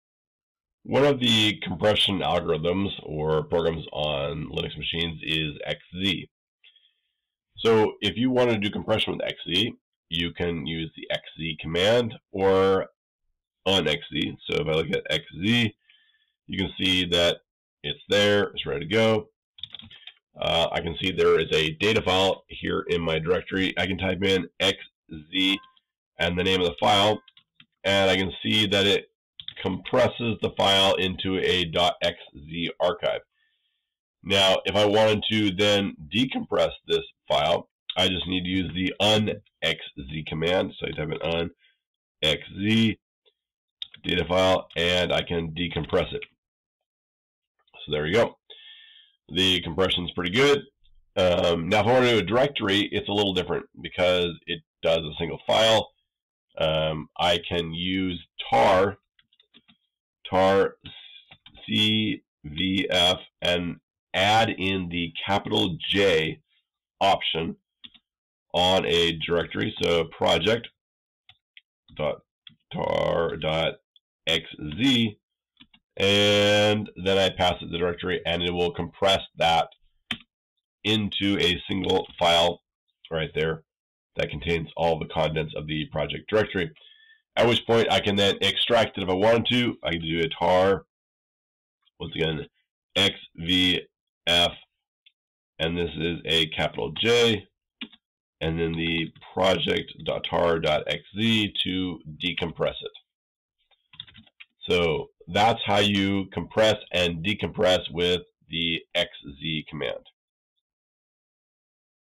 Compression with xz on Linux machines
Shows how to compress and decompress a single file using the xz and unxz utilities.  Also, shows how to compress and decompress a directory tree using the xz compression and the tar command.